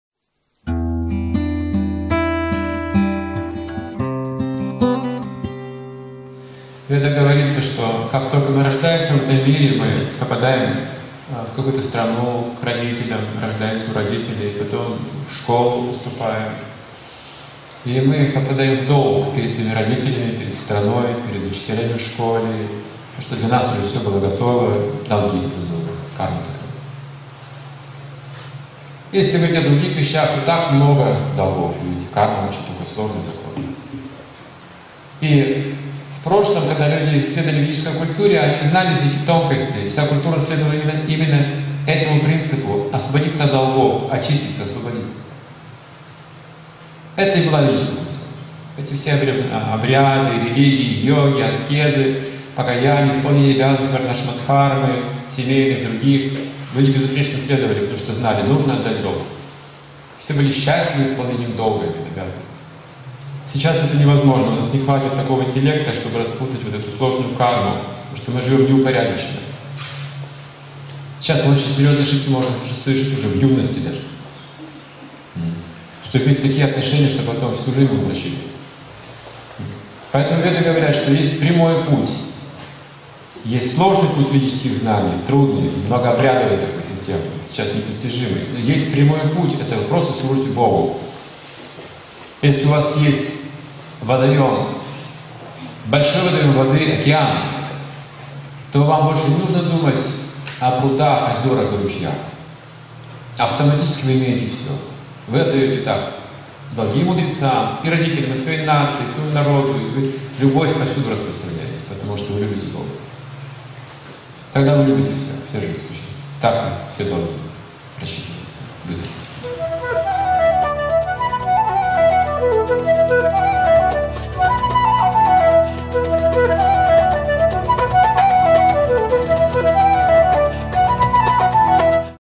315_Как рассчитаться с долгами, которые мы получаем при рождении
При рождении мы сразу получаем долги: перед родителями, перед страной, перед народом. В настоящее время рассчитаться за все долги человек может, просто служа Богу.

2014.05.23, Россия, Челябинск, Обратная сторона иллюзии